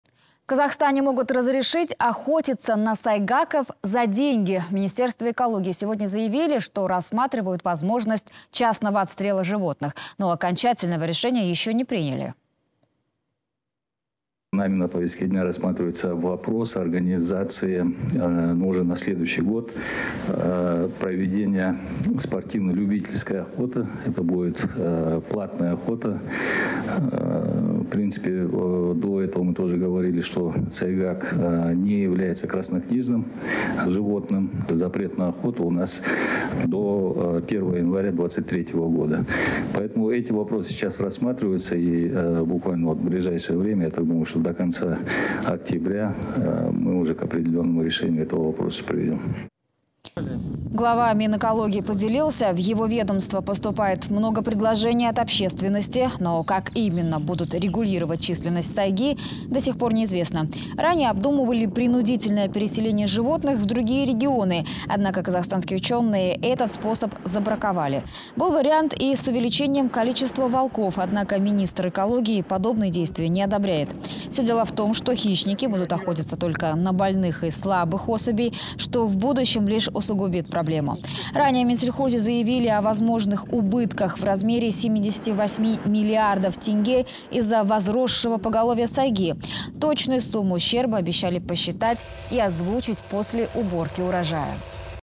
В Минэкологии обдумывают возможность платной охоты на сайгу
В Казахстане могут разрешить охотиться на сайгаков за деньги. В Министерстве экологии сегодня заявили, что рассматривают возможность частного отстрела животных. Но окончательного решения еще не приняли.